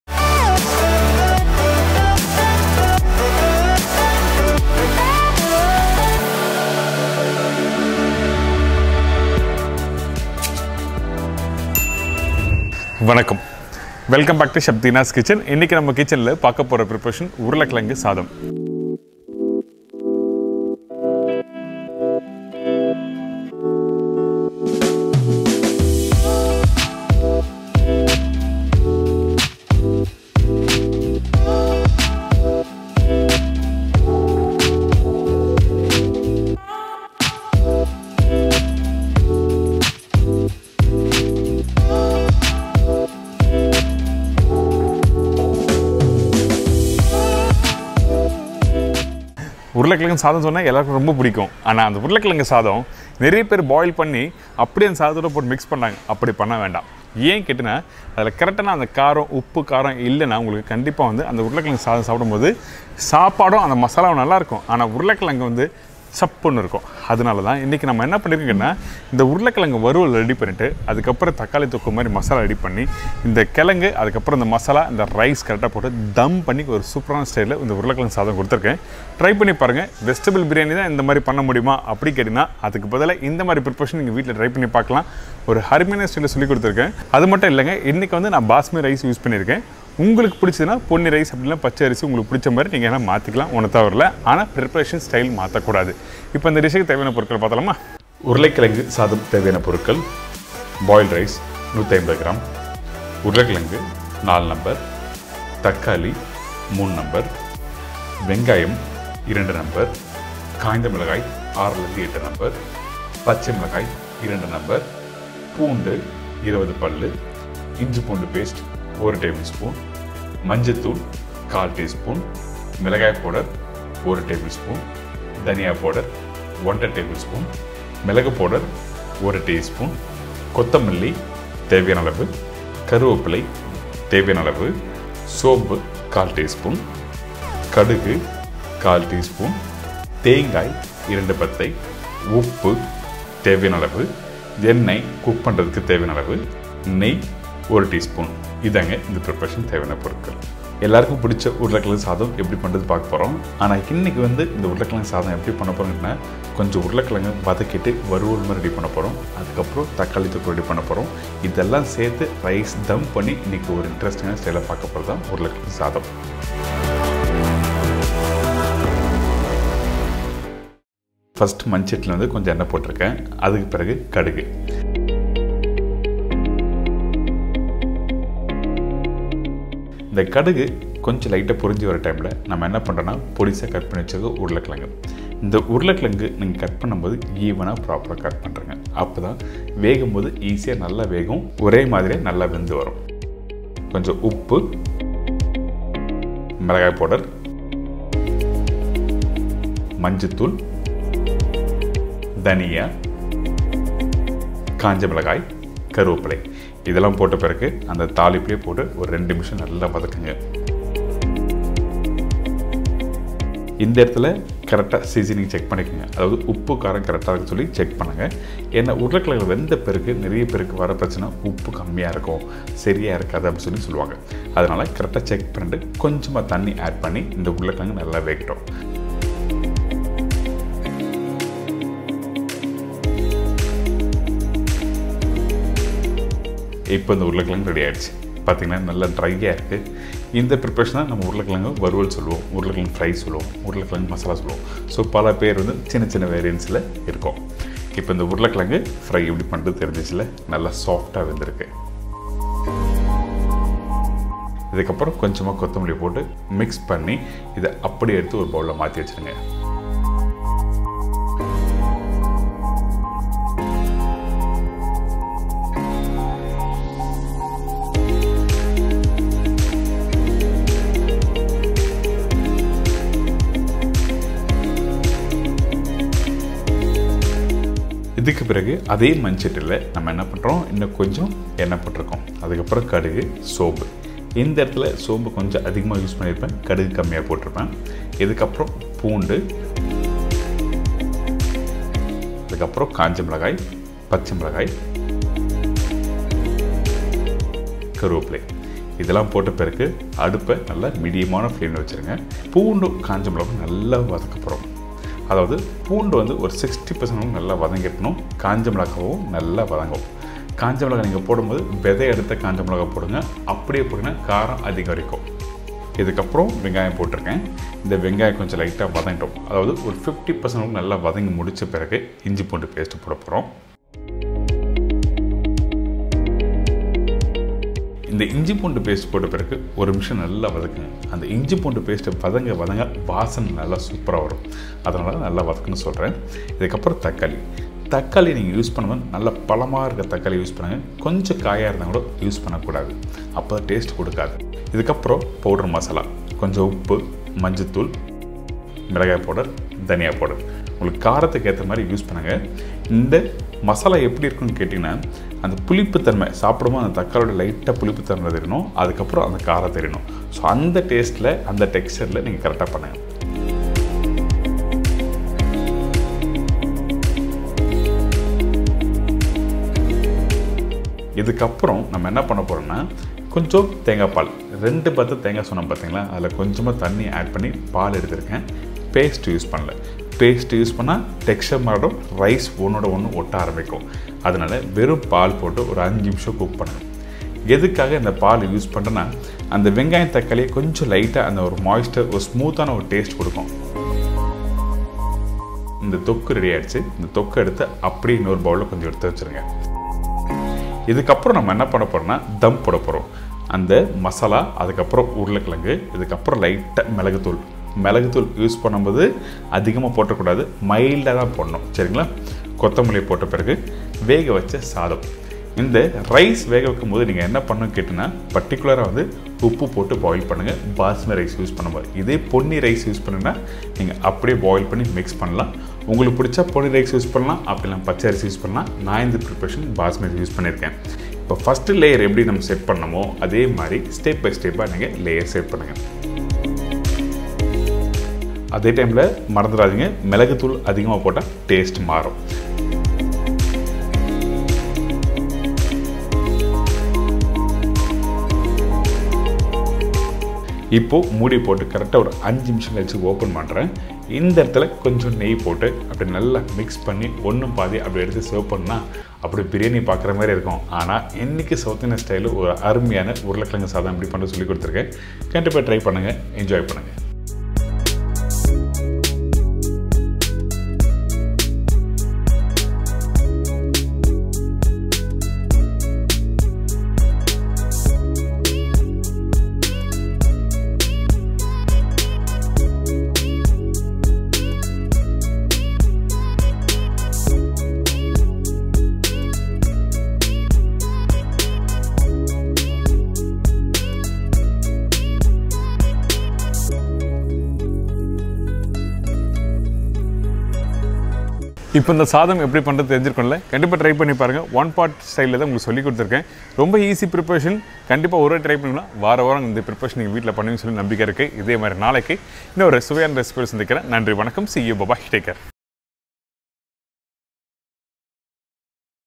உருளைக்கிழங்கு சாதம் | Urulai Kizhangu Sadham | Potato Recipes | CDK #436 | Chef Deena's Kitchen
Boiled Rice - 150g
Potato ( Medium Size ) - 4 No's
Dry Red Chilli - 6 to 8 No's
Green Chilli - 2 No's
Garlic - 20 No's
Coriander Powder - 1 1/2 tbsp